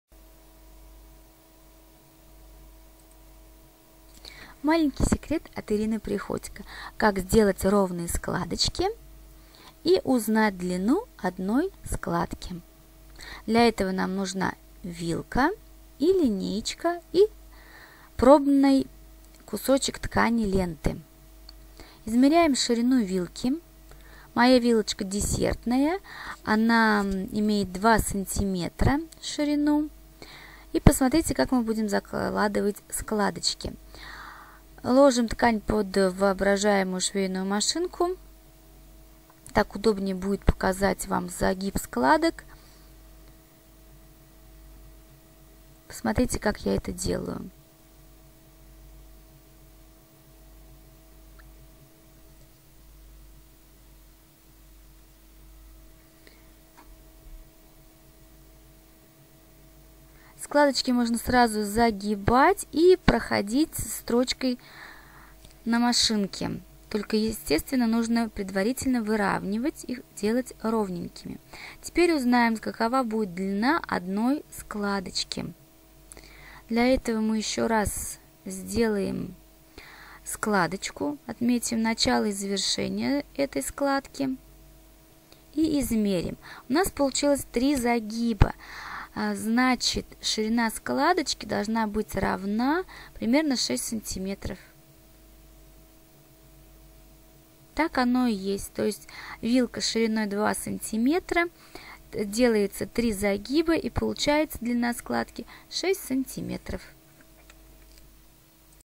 складочки вилкой
Складочки с помощью вилки
Специально для МК "Платье "Май" (это для новеньких)
Весь мастер-класс по хештэгу#платьеМай_мкШиК

Вы можете публиковать свой процесс пошива платья с пометкой " Шью платье с @vykroykiris